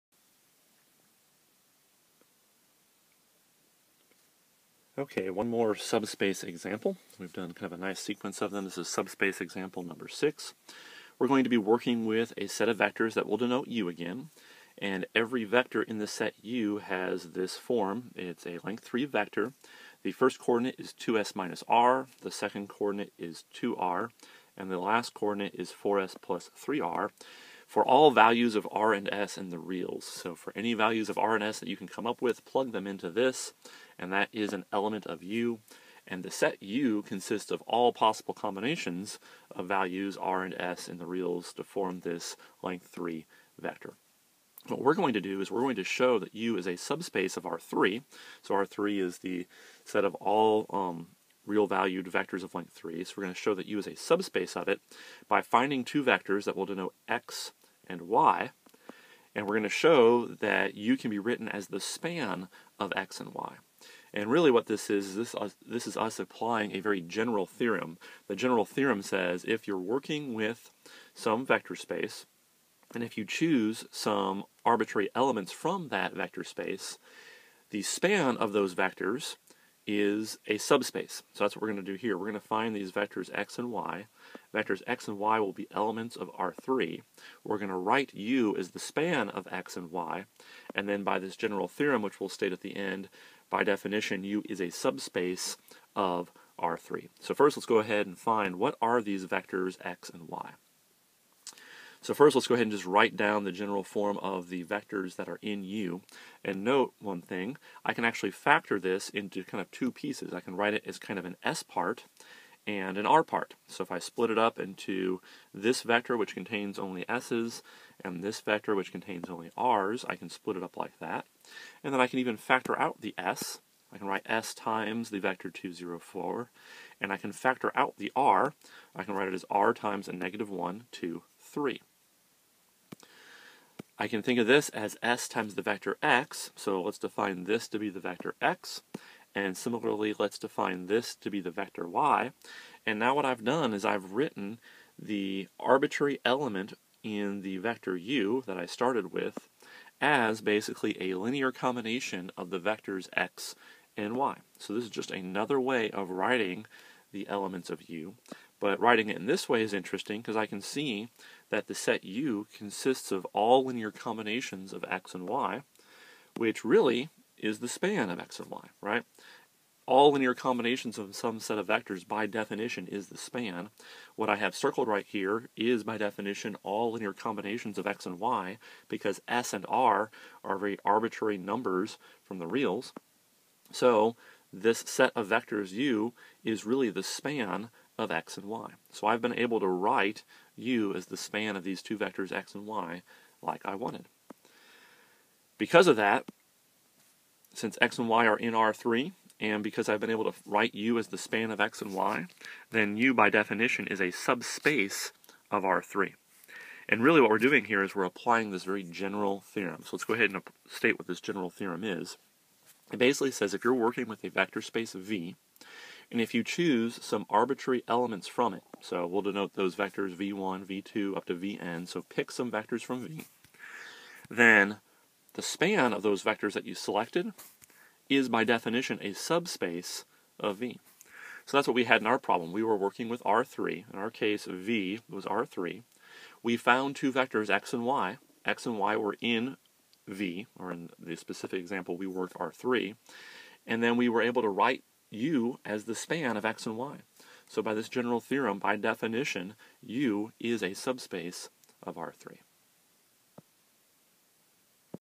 Linear Algebra Example Problems - Subspace Example #6
This examples uses a general theorem to show that a set of vectors U is indeed a subspace of R3.  The general theorem says that the span of any vectors from a vector space is a subspace.

To use this theorem we find a way to write the elements of U as a linear combination of vectors from R3.  Thus, the set U is the span of some vectors from R3, and by the general theorem, U is a subspace of R3.